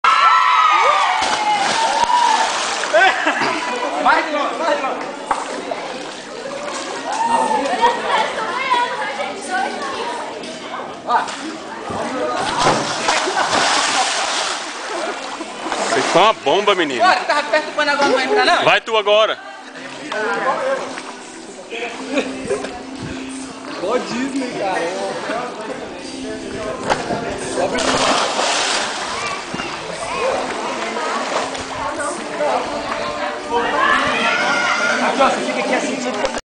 Jovens Batista Apascentar DF Churrasco de Despedida da Amanda - Mergulho dos 3 15/03/09
Nossos 3 melhores atletas em mergulho fenomenal, Maisena, Claudinho e Allan!